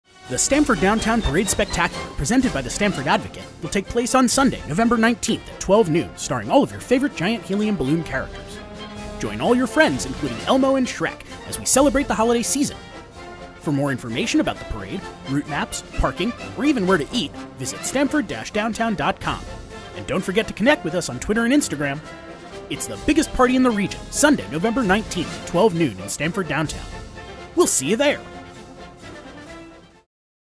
2017 Stamford Downtown Parade Spectacular Commercial
The Stamford Downtown Parade Spectacular is one of the largest helium balloon parades in the country, featuring everyone’s favorite giant balloon characters, award winning marching bands and fabulous floats.  Fun for the whole family!